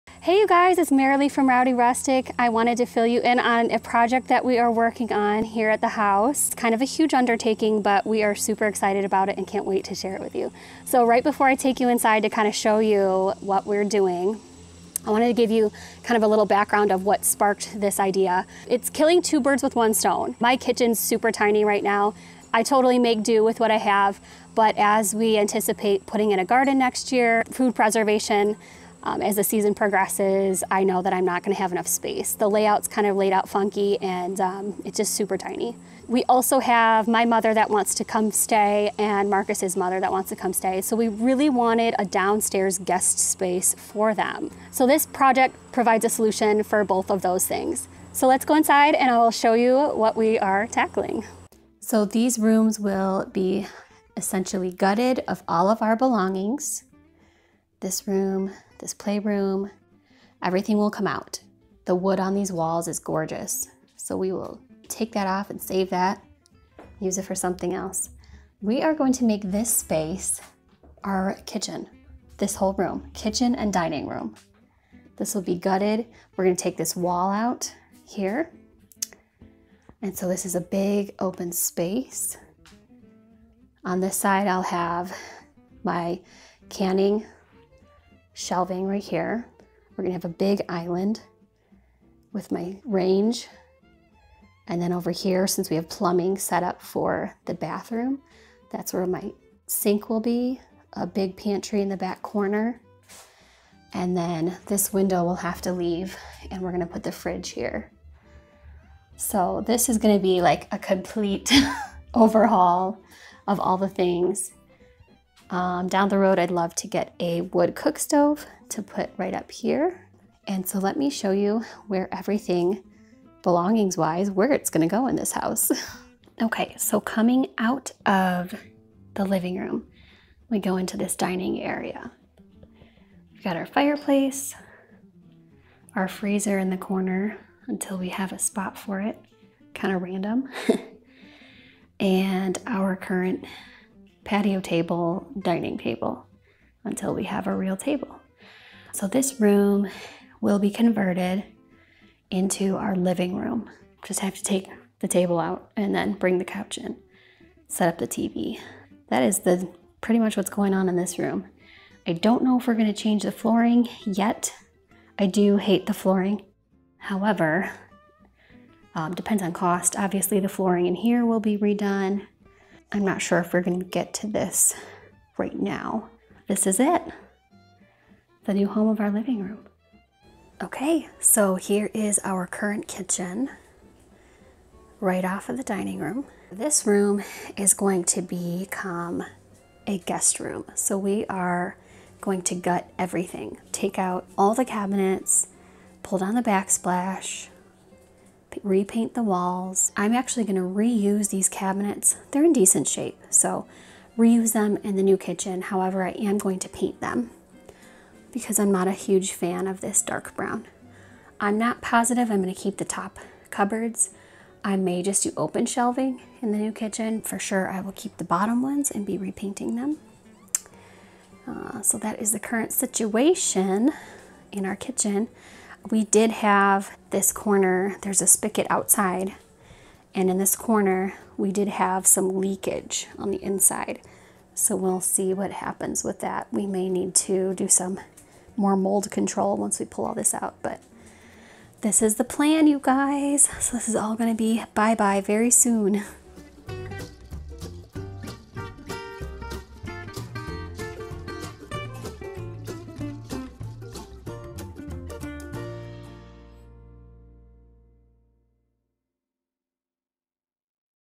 Homestead Rowdy Renovation
We continue challenging ourselves by learning new skills and establishing ourselves on our homestead.

Part of that process is renovating the inside of our home to facilitate our new lifestyle.

More updates to come on our renovation and many other things going on around the Rowdy Rustic homestead.

Let us know what you thought of the video down in the comments.

MUSIC CREDIT